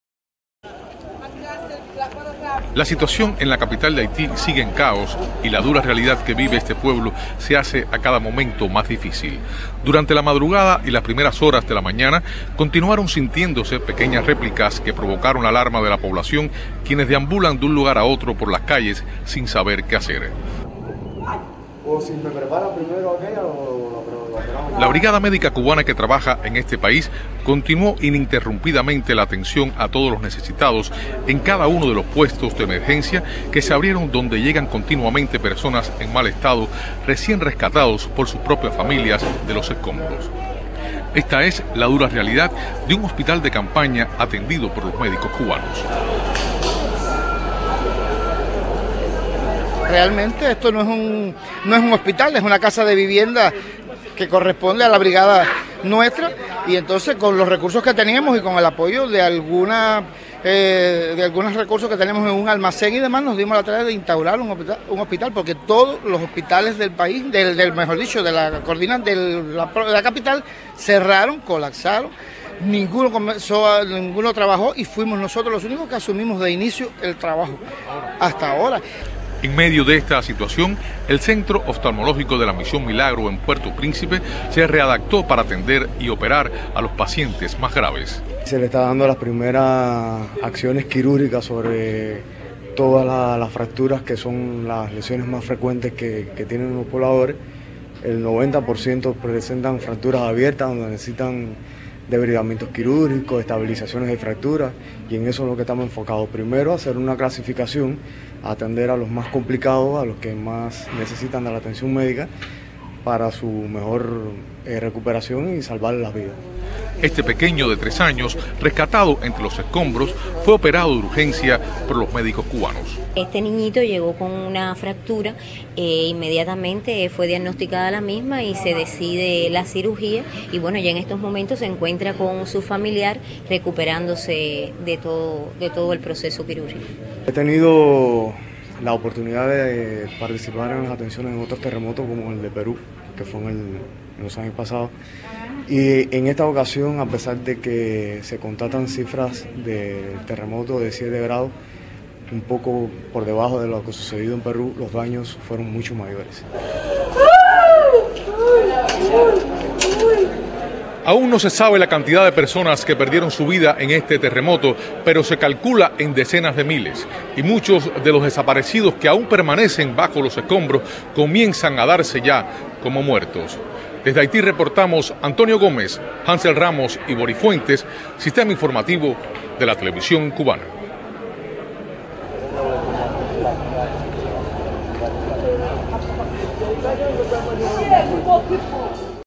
En Haití atienden médicos cubanos en hospitales de campaña
El drama crece en Haití. Miles  de personas perdieron sus casas, mientras millones carentes de agua, electricidad y gas deambulan sin saber a dónde ir. En medio de ese panorama desolador, la brigada médica cubana continúa brindando ayuda en improvisados hospitales de campaña.